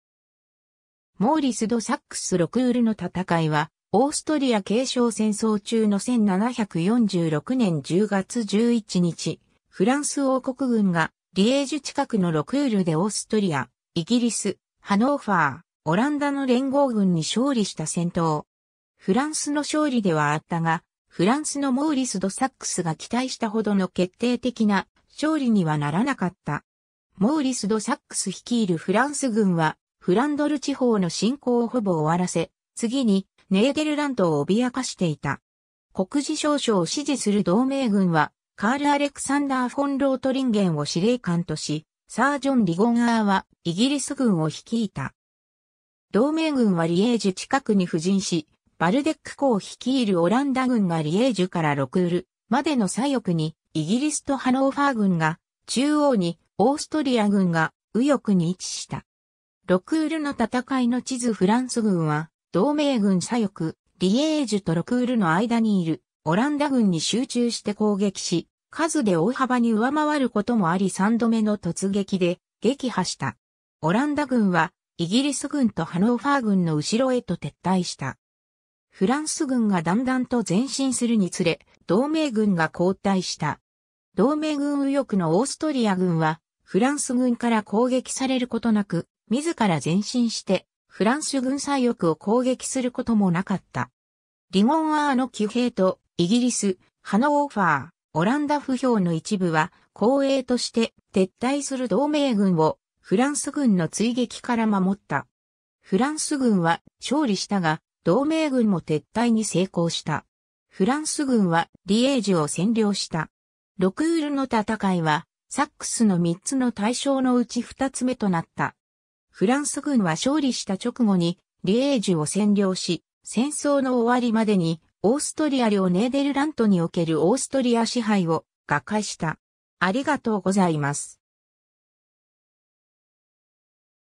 ロクールの戦い
モーリス・ド・サックス
モーリス・ド・サックス ロクールの戦い（ロクールのたたかい、フランス語: Bataille de Rocourt）は、オーストリア継承戦争中の1746年10月11日、フランス王国軍がリエージュ近くのロクールでオーストリア、イギリス、ハノーファー、オランダの連合軍に勝利した戦闘。
フランスの勝利ではあったが、フランスのモーリス・ド・サックスが期待したほどの決定的な勝利にはならなかった。
 モーリス・ド・サックス率いるフランス軍はフランドル地方の侵攻をほぼ終わらせ、次にネーデルラントを脅かしていた。
国事詔書を支持する同盟軍はカール・アレクサンダー・フォン・ロートリンゲンを司令官とし、サー・ジョン・リゴンアーはイギリス軍を率いた。
同盟軍はリエージュ近くに布陣し、ヴァルデック侯率いるオランダ軍がリエージュからロクールまでの左翼に、イギリスとハノーファー軍が中央に、オーストリア軍が右翼に位置した。
 ロクールの戦いの地図 フランス軍は同盟軍左翼、リエージュとロクールの間にいるオランダ軍に集中して攻撃し、数で大幅に上回ることもあり3度目の突撃で撃破した。
オランダ軍はイギリス軍とハノーファー軍の後ろへと撤退した。
フランス軍がだんだんと前進するにつれ、同盟軍が後退した。
同盟軍右翼のオーストリア軍はフランス軍から攻撃されることなく、自ら前進してフランス軍左翼を攻撃することもなかった。
リゴンアーの騎兵とイギリス、ハノーファー、オランダ歩兵の一部は後衛として、撤退する同盟軍をフランス軍の追撃から守った。
フランス軍は勝利したが、同盟軍も撤退に成功した。
フランス軍はリエージュを占領した。
 ロクールの戦いは、サックスの3つの大勝のうち2つ目となった。
フランス軍は勝利した直後にリエージュを占領し、戦争の終わりまでにオーストリア領ネーデルラントにおけるオーストリア支配を瓦解した。